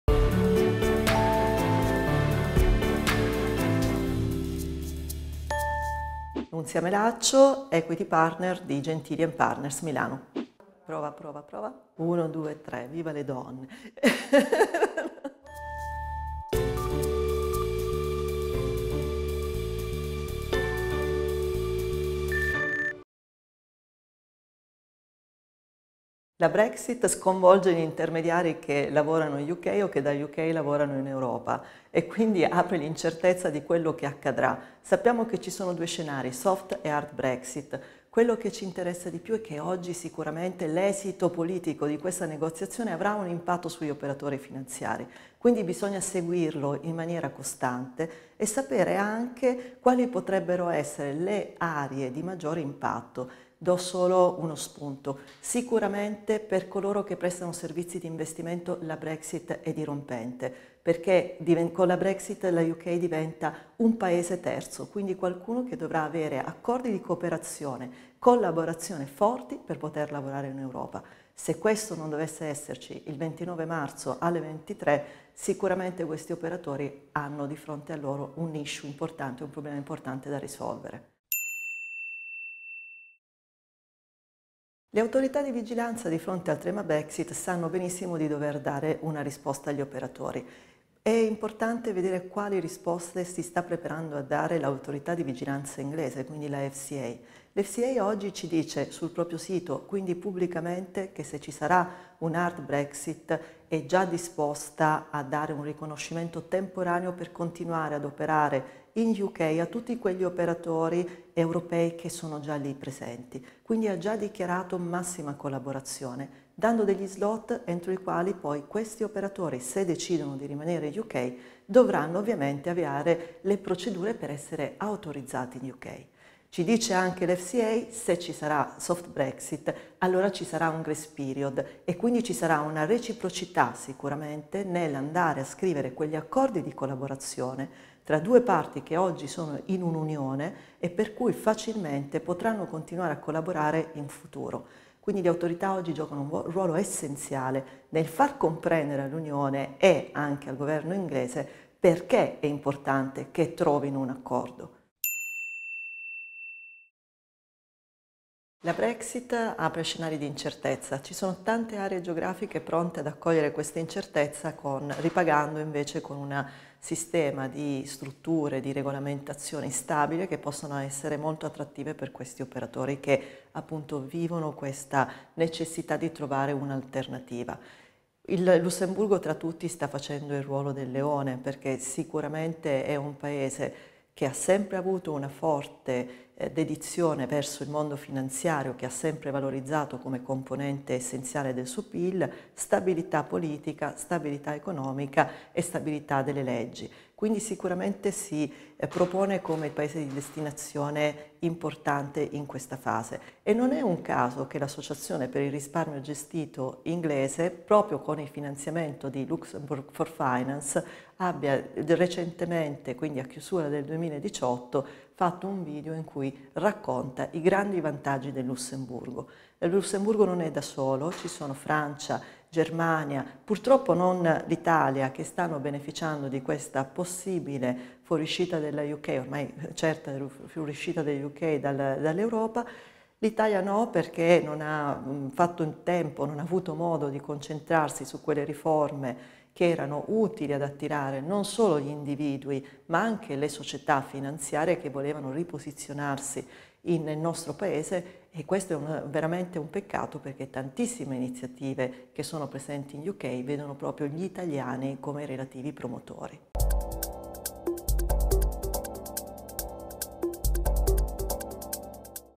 Food for Thought - Brexit, il futuro degli intermediari
Quali scenari Brexit apre per gli intermediari e come si stanno preparando le autorità di vigilanza? Risponde Nunzia Melaccio, equity partner di Gentili & Partners